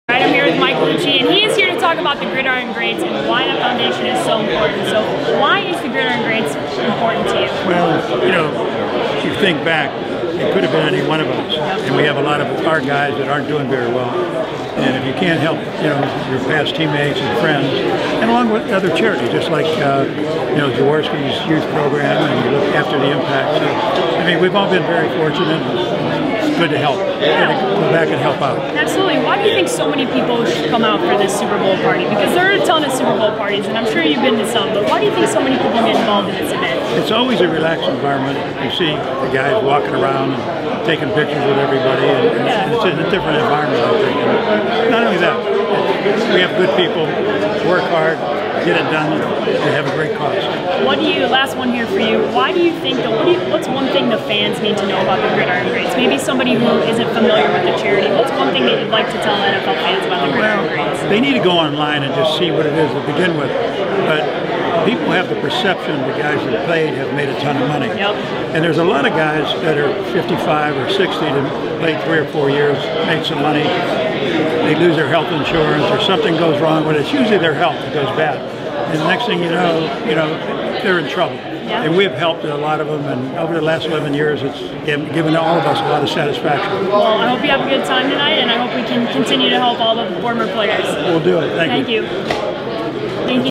Mike Lucci
Interview with Mike Lucci from the Ditka Jaws party.